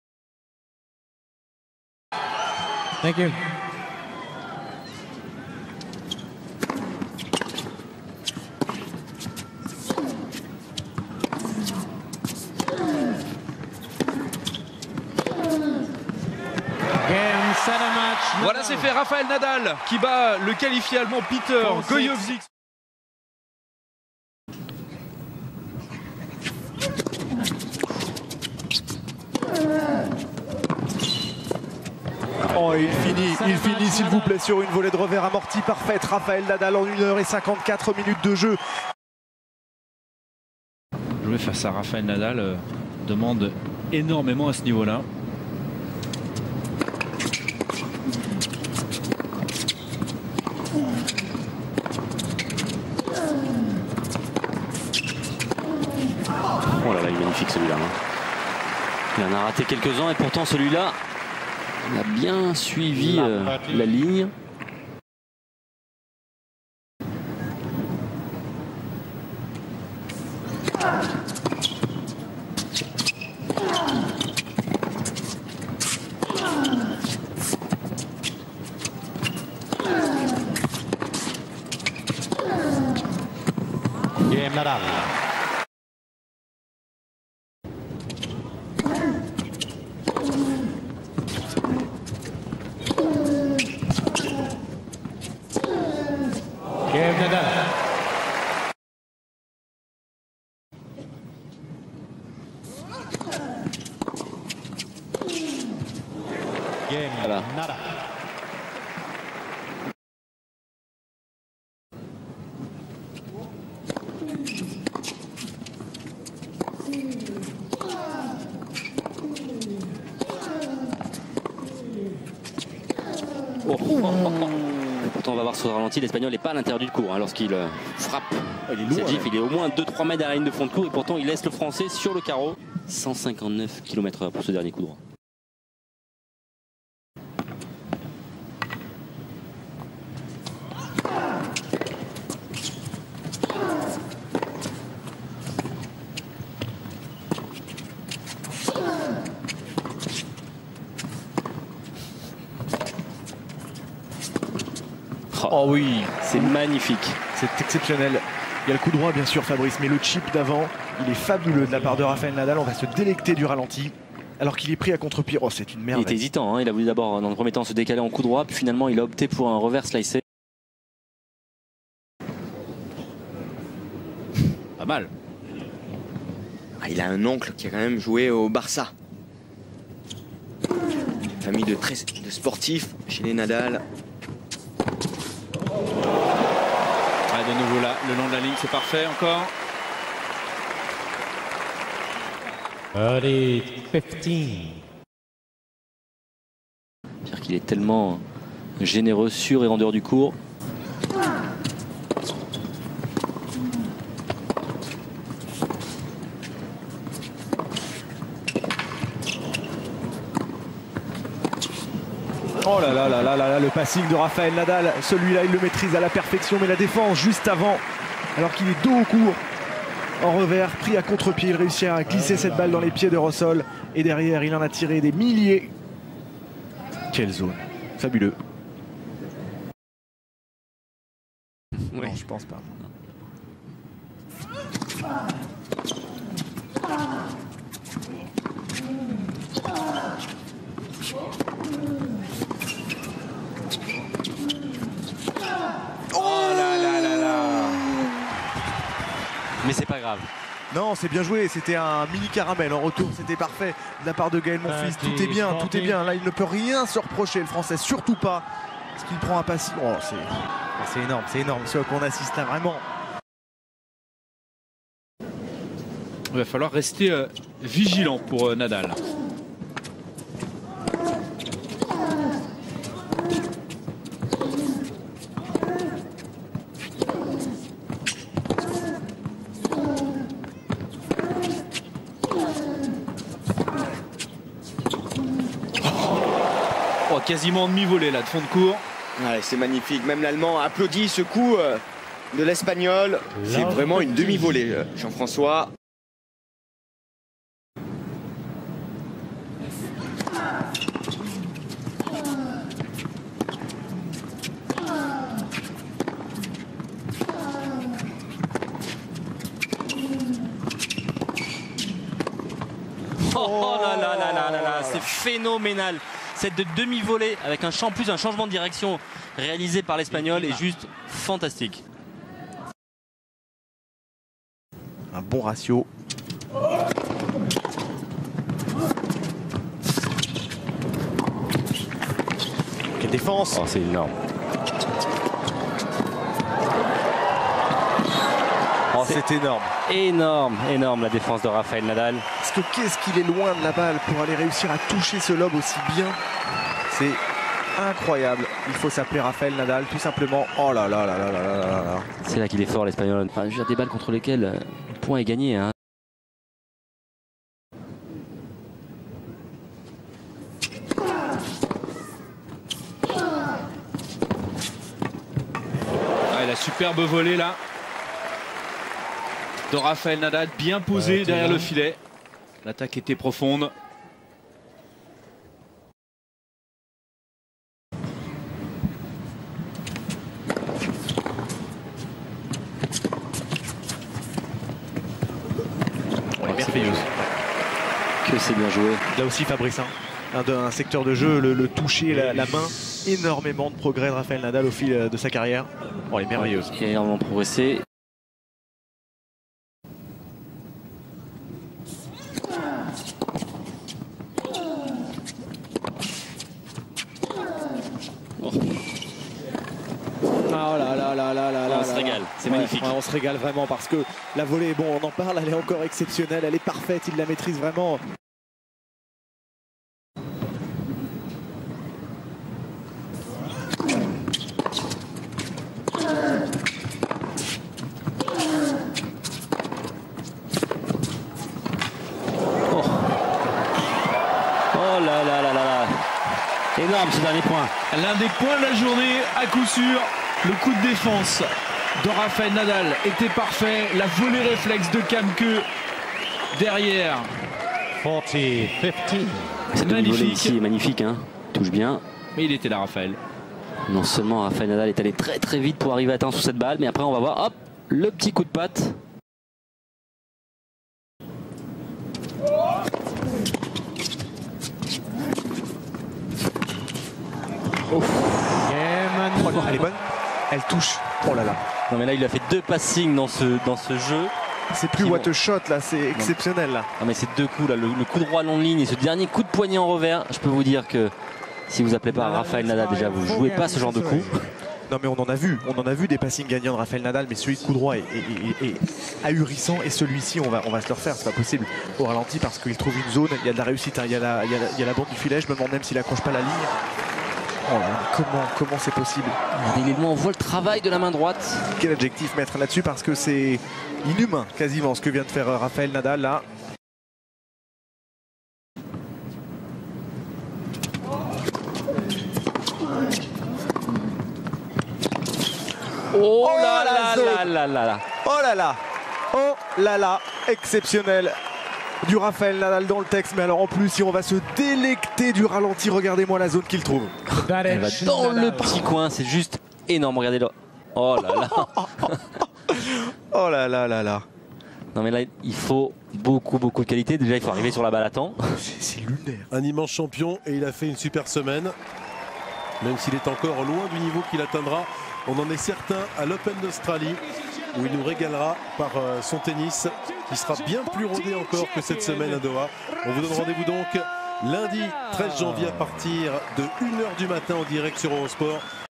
Doha 2014 : Rafael Nadal Best Points TOP 20 HD - Qatar Open
Mon TOP 20 des meilleurs points de Rafael Nadal à l'Open de Doha 2014. My TOP 20 Best Points of Nadal in the Doha Open 2014 against Lukas Rosol, Tobias Kamke, Ernests Gulbis, Peter Gojowczyk and Gaël Monfils.